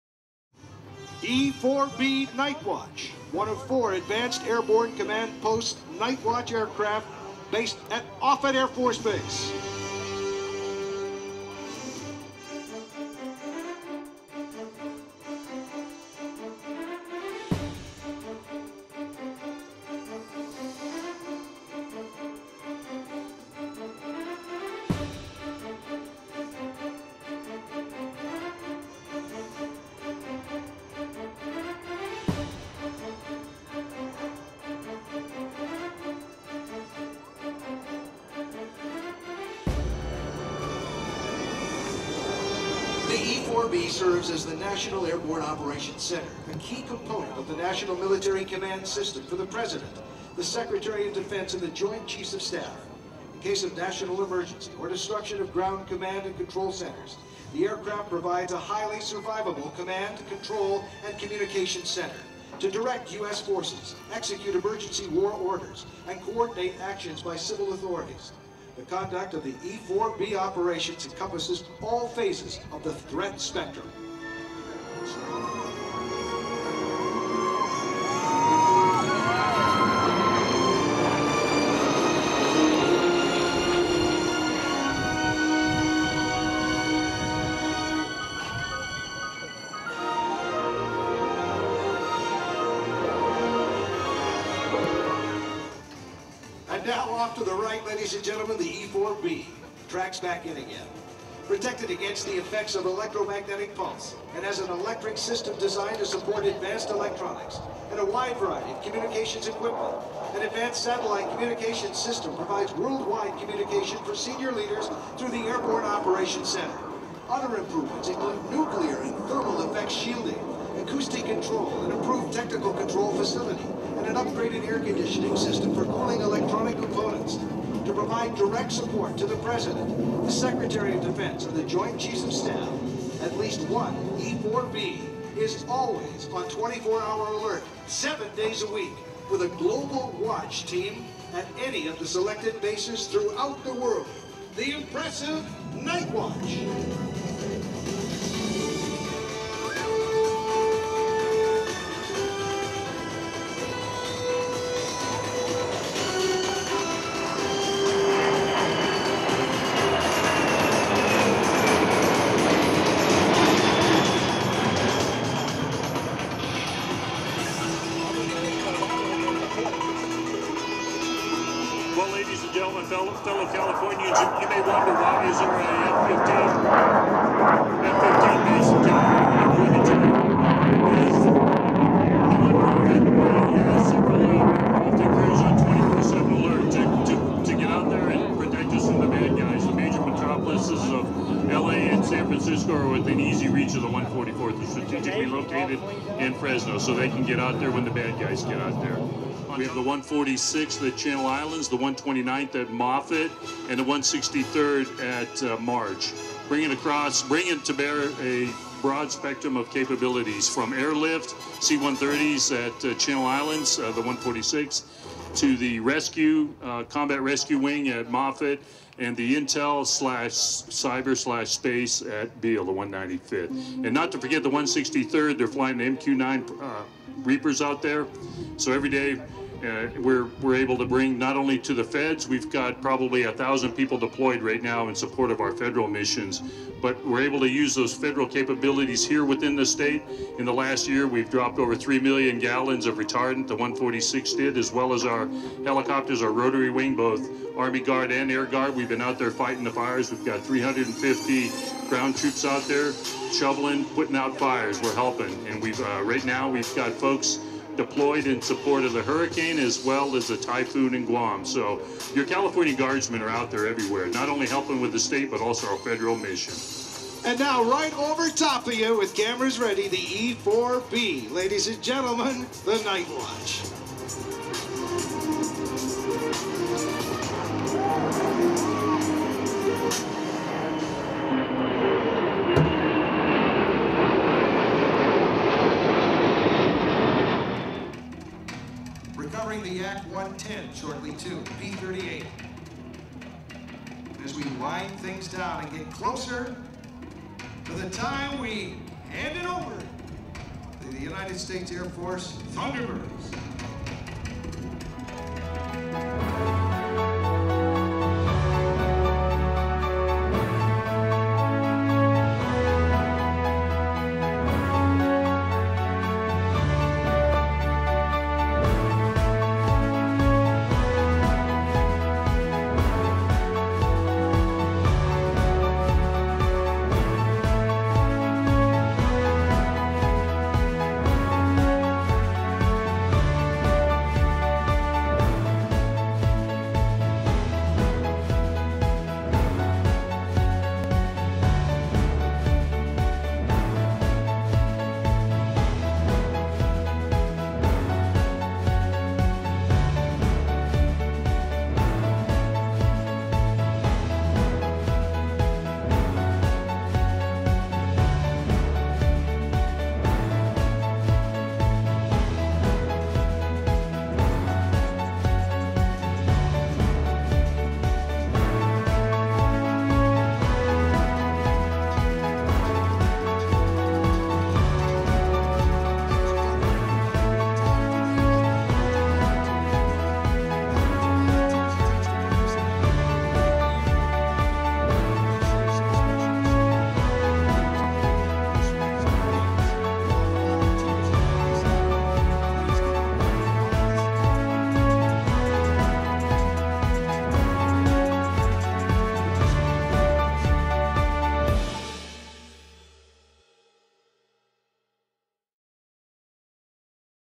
Doomsday 747 E-4B Nightwatch at the California Capital Airshow
The Boeing E-4 Advanced Airborne Command Post, the current "Nightwatch" aircraft, is a strategic command and control military aircraft operated by the United States Air Force (USAF). The E-4 series was specially modified from the Boeing 747-200B for the National Emergency Airborne Command Post (NEACP) program. The E-4 serves as a survivable mobile command post for the National Command Authority, namely the President of the United States, the Secretary of Defense, and successors. The four E-4Bs are operated by the 1st Airborne Command and Control Squadron of the 595th Command and Control Group located at Offutt Air Force Base, near Omaha, Nebraska. An E-4B when in action is denoted a "National Airborne Operations Center"